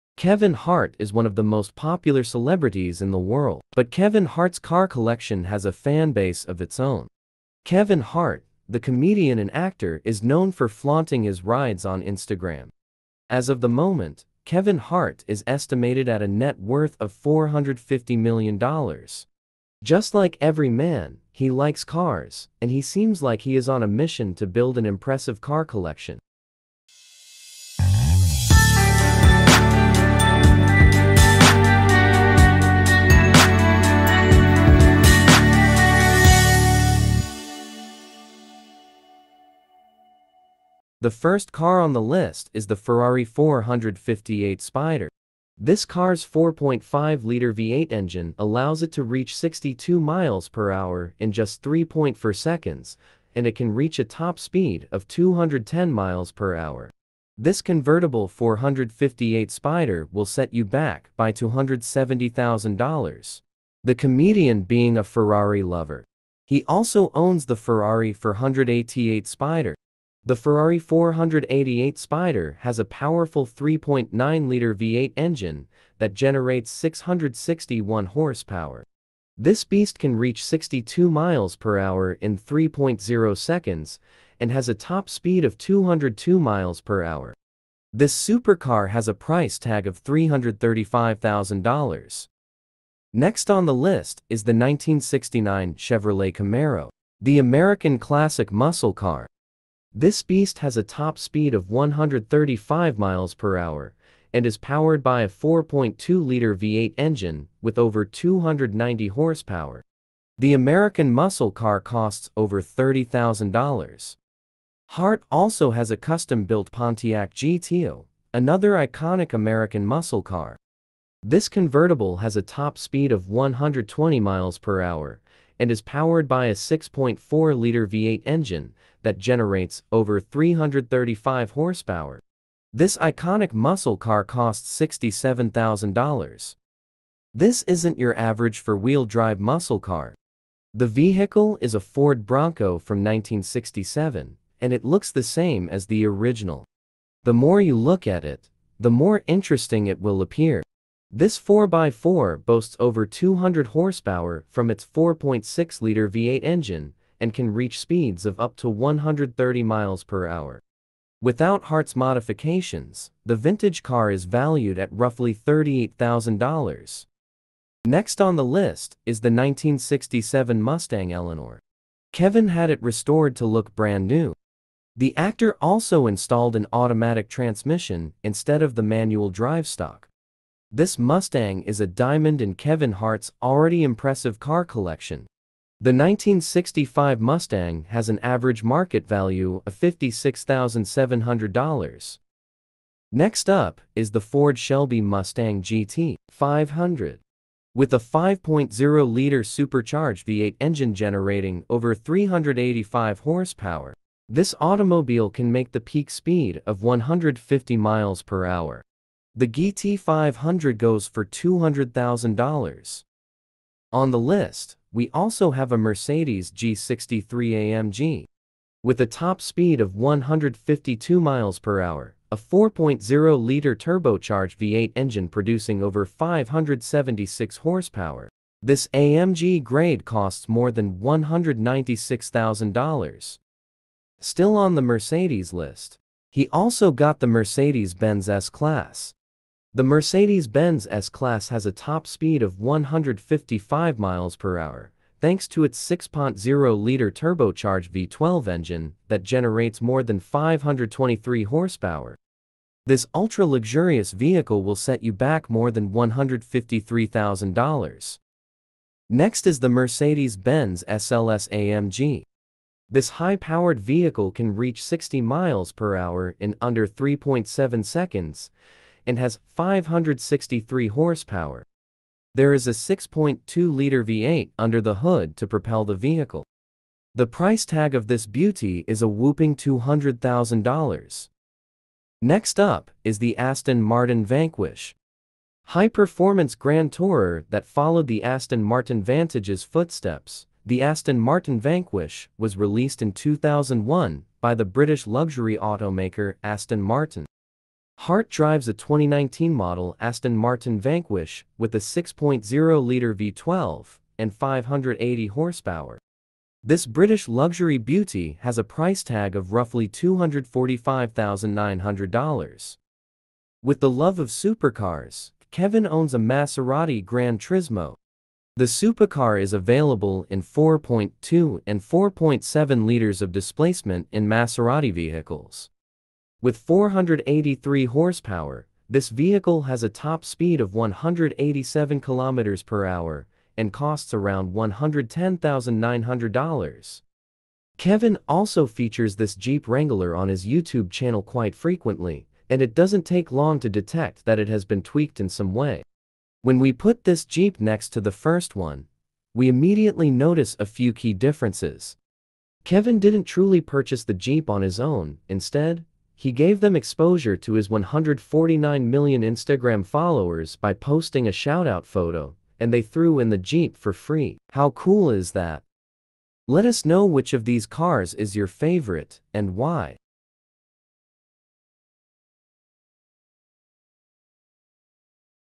A Look inside Kevin Hart’s fabulous Car Collection.(muscle cars,Vintage,luxury and sports cars)
A Look inside Kevin Hart’s fabulous Car Collection.(muscle cars,Vintage cars,luxury cars and sports cars) of 2023.

Kevin Hart is one of the most popular celebrities in the world. But Kevin Hart’s car collection has a fan base of its own.

Kevin Hart, the comedian, and actor is known for flaunting his rides on Instagram. As of the moment, Kevin Hart is estimated at a net worth of $450 million. Just like every man, he likes cars, and he seems like he is on a mission to build an impressive car collection. Curious about Kevin hart’s car collection? Way.com has you covered. Read on to know the list of cars he owns.

Kevins Hart’s fabulous Car Collection

Kevin Hart’s networth